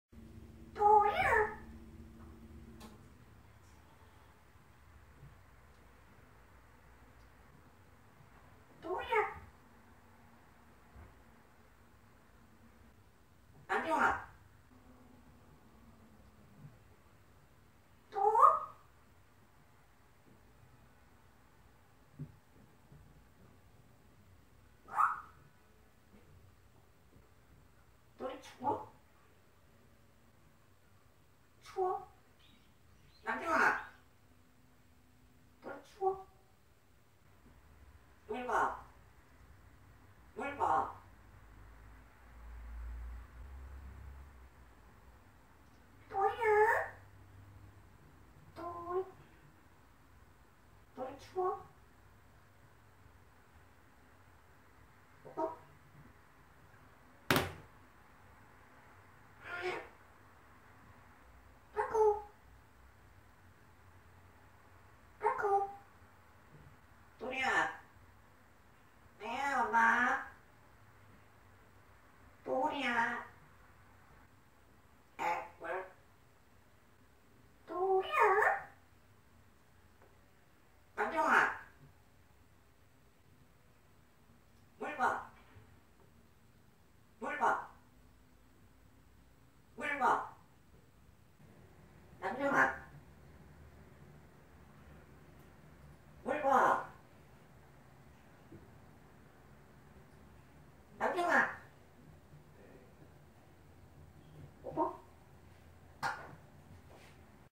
연구대상우똘이 감사인사드립니다
염려해주셔서감사드립니다 악풀보다 선풀이 많아서 감사합니다
우리 똘이는 천재앵무새 회색앵무 우똘이 입니다